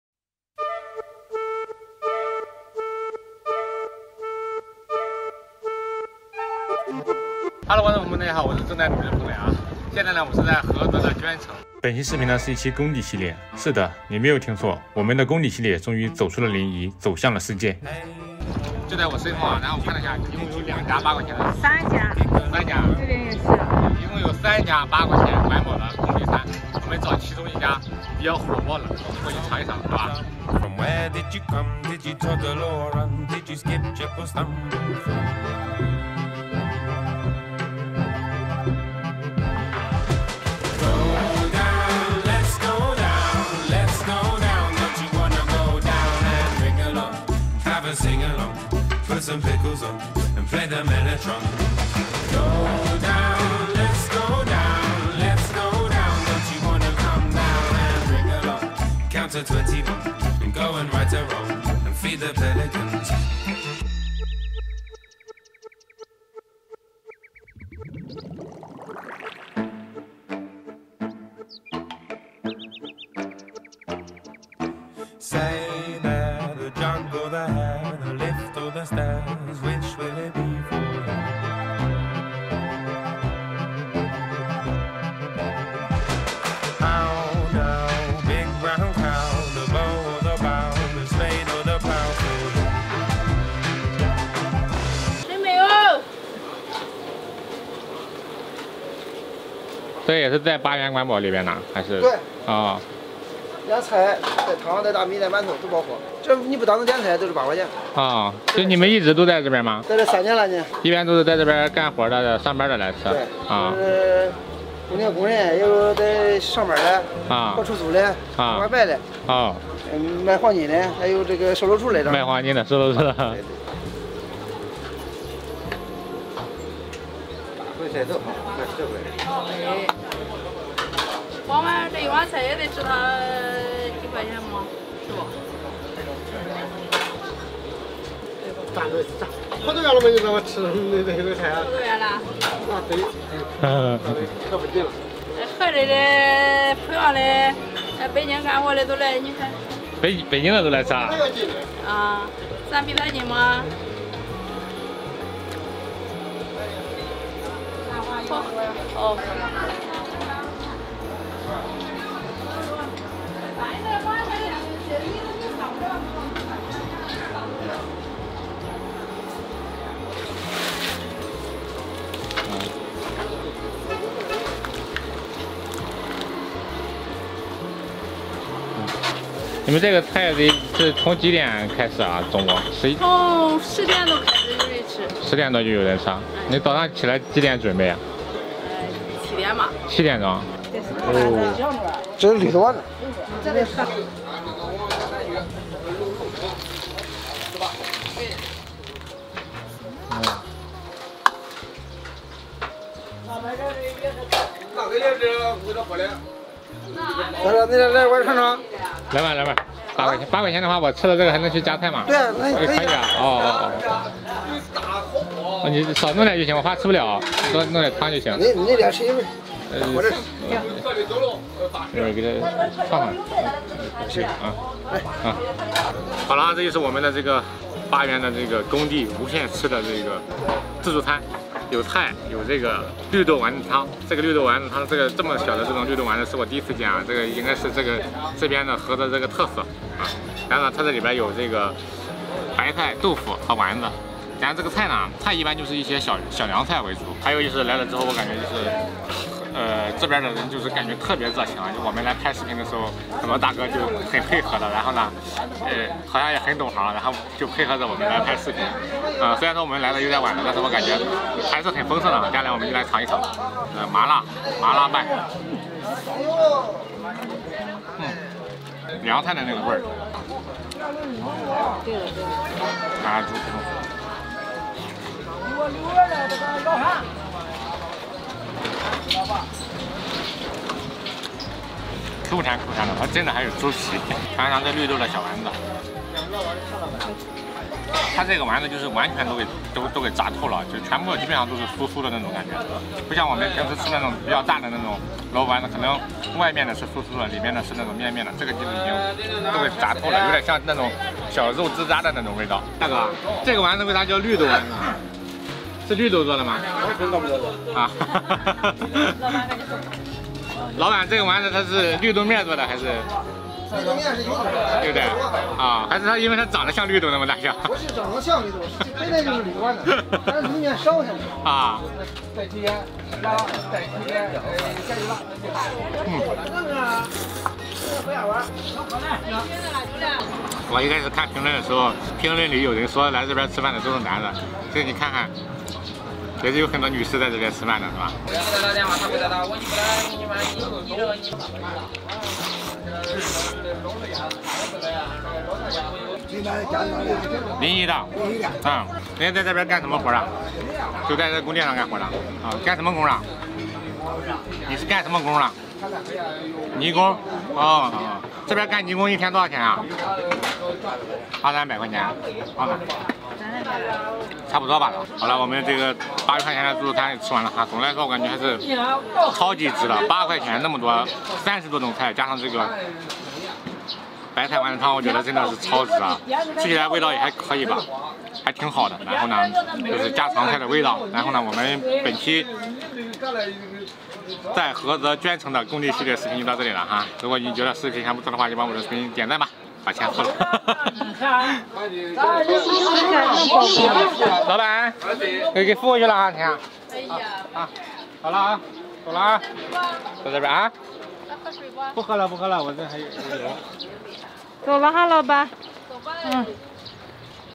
山东实在老板工地旁搞8元自助，30多种菜不限量，一锅丸子汤吸引人跨省来吃
本期工地系列来到了山东菏泽鄄城，大锅炖绿豆丸子汤相当美味，还有三十多种菜无限续。